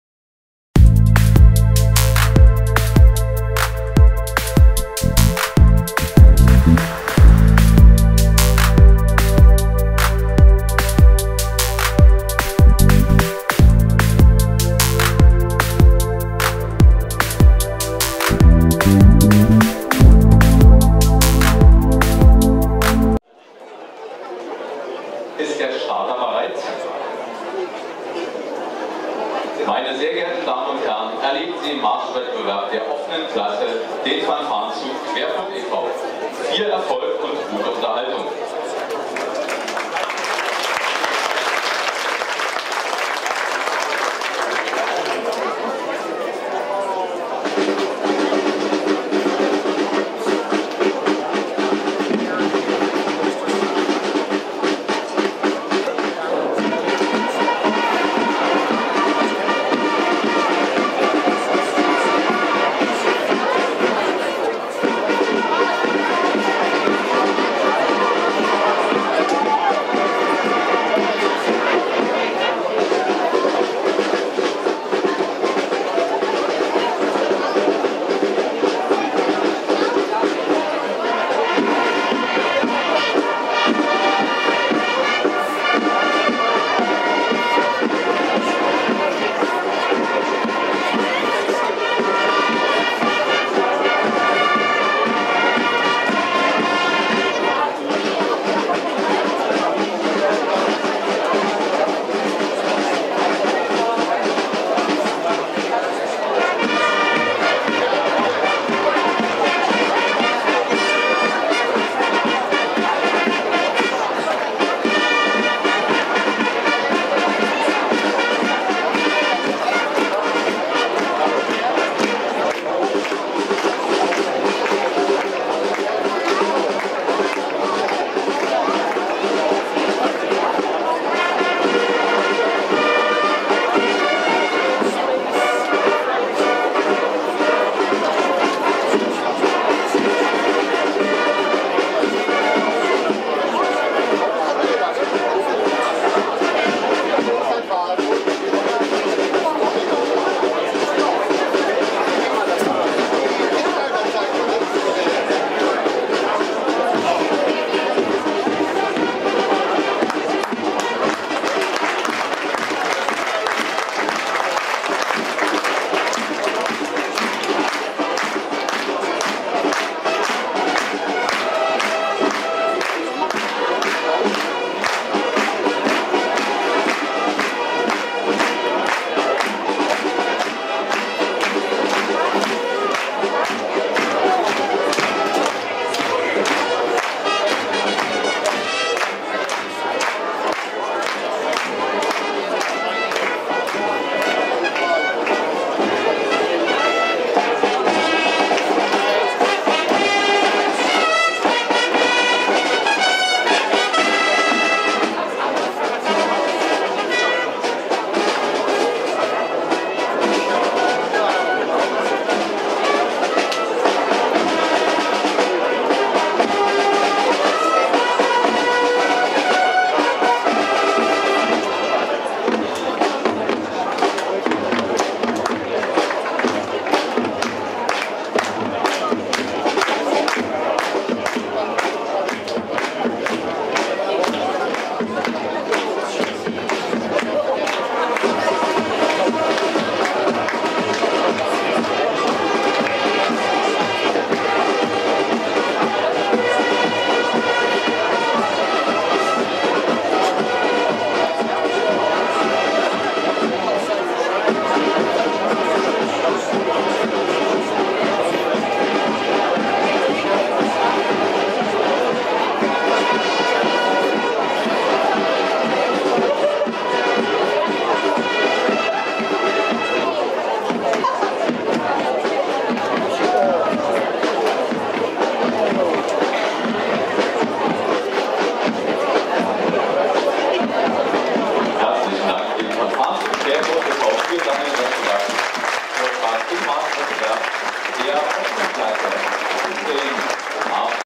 Fanfaronade 2016 3D (12) Marsch OK - Fanfarenzug Querfurt
26. Fanfaronade des MTB in Neustrelitz - was Zuschauer und Wertungsrichter zu sehen und zu hören bekamen.
3D-Rec.: JVC GS-TD1' - mit Zusatzmikro  JVC MZ-V10EU
Created with Cyberlink PowerDirector14 DirectorSuite4